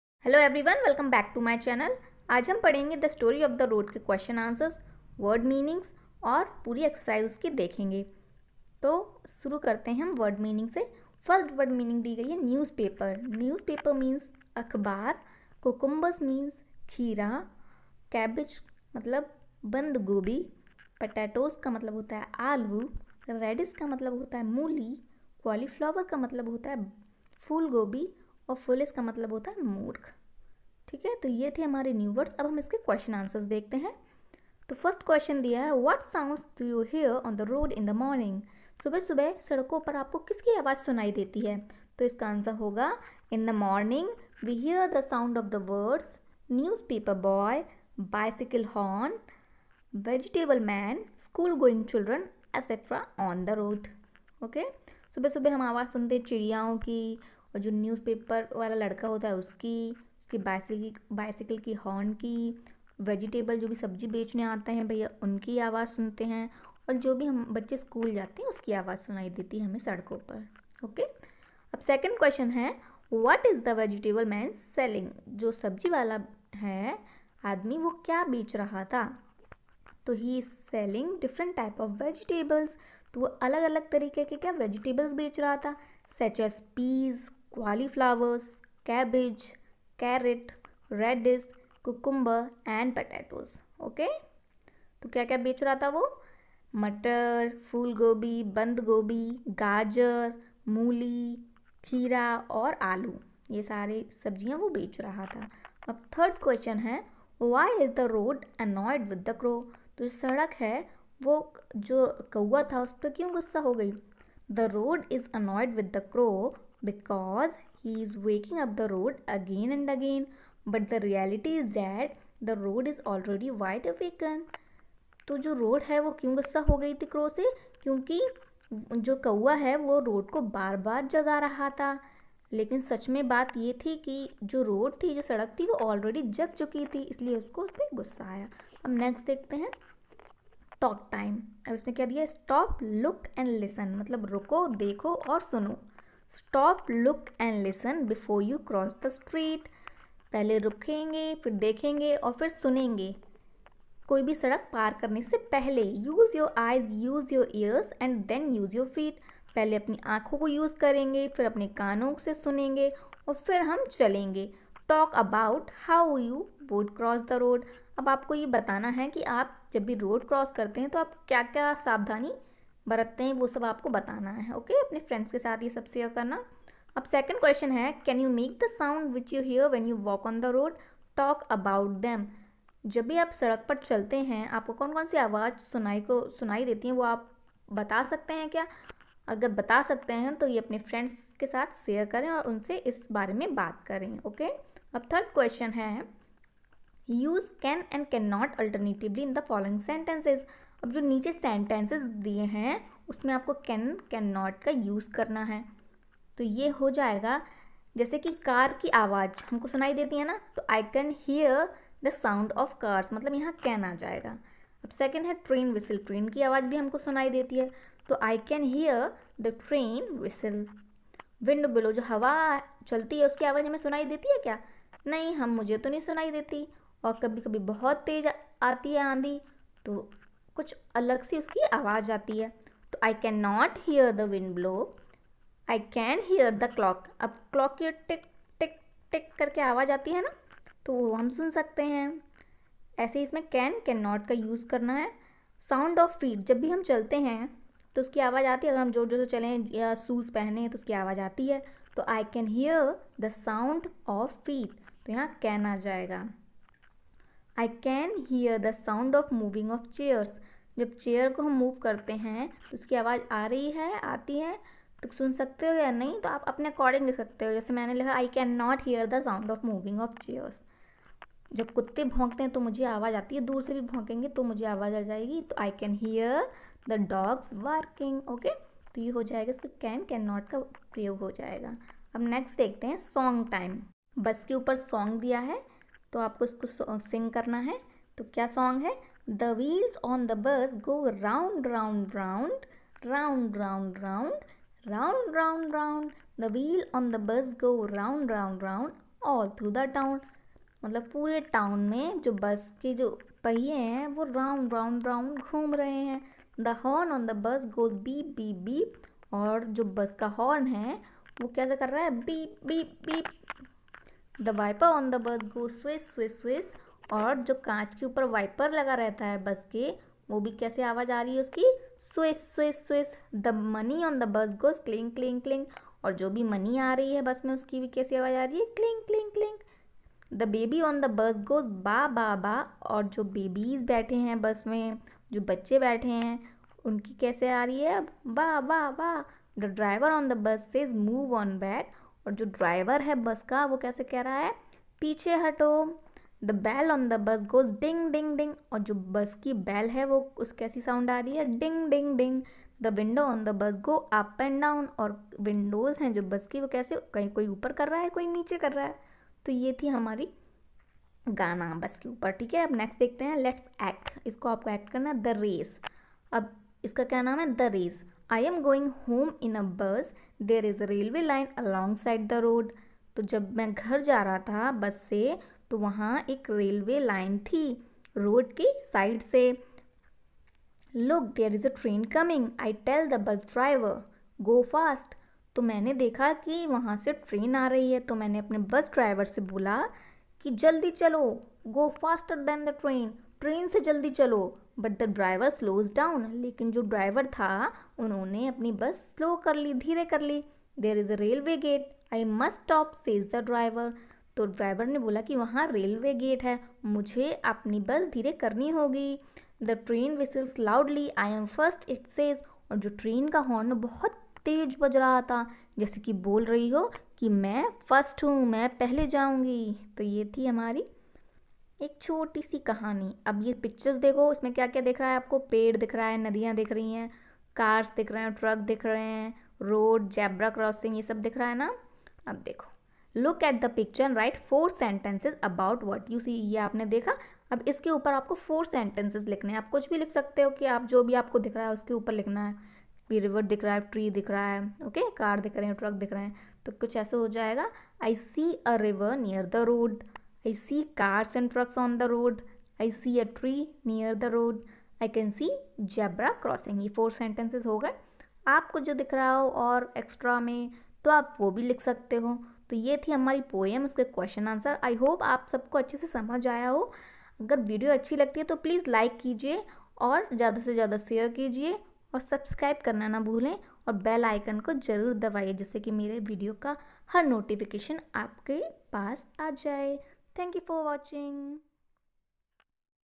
class 3 (the story of the road) Question Answer word meaning solution
hello everyone, aj me apko the story of the road k question Answer word meaning exercise sab solve karege, I hope apko sab ache se samajh aaye.
if this video is helpful then please do